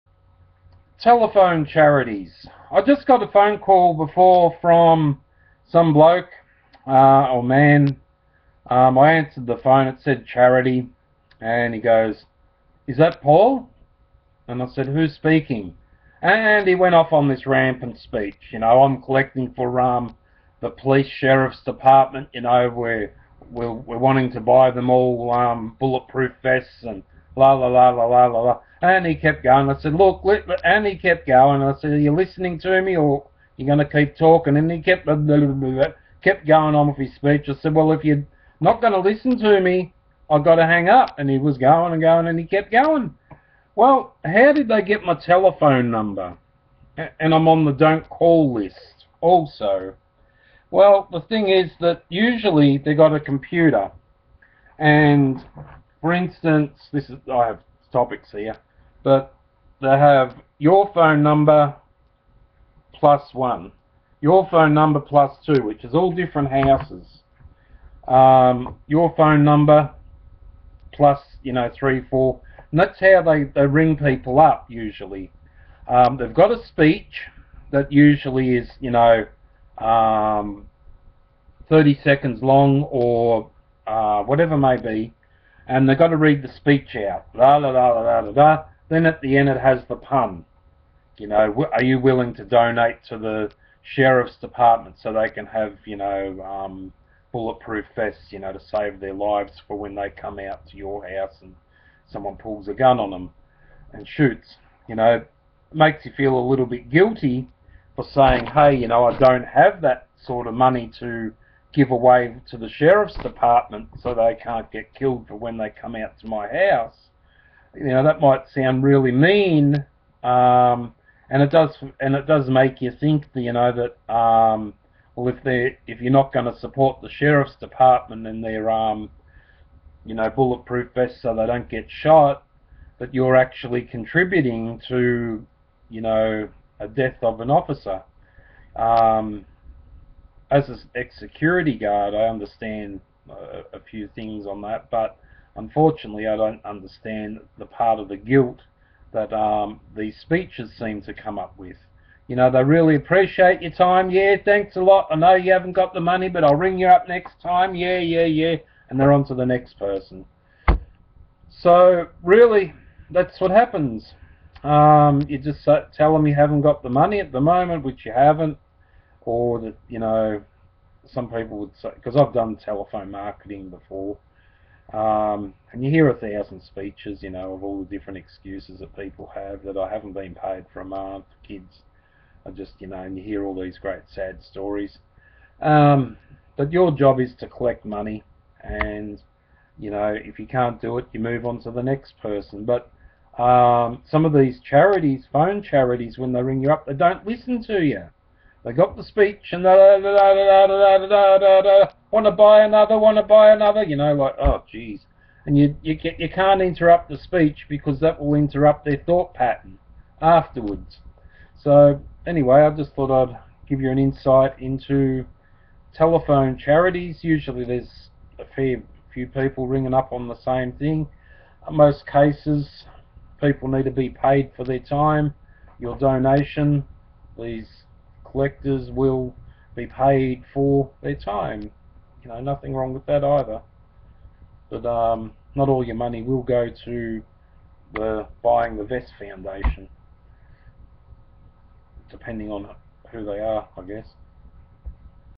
Bullet Proof Vests 4 Police...Telephone Charitys How They Work, Or How They Collect Money?
I got a call today from a charity, the person asked if my name was Paul, I said who are you?
Then the person when off on a long speech about how they are collecting money for the police so they can own a bullet proof vest... I tried to interrupt the phone sales man but he kept at the lines he was reading on the computer.

So I hung up, these type of calls are made to make you feel guilty to not donate to save a police mans life by helping buy a bullet proof vest.

These calls are made in a way if you do not donate to save a police mans life through a bullet proof vest they may not turn up too your house in time's of safety or need of a 911 assistance call. Just my opinion on some of the calls I have answered here in America over the years.

Even that you are on the do not call list.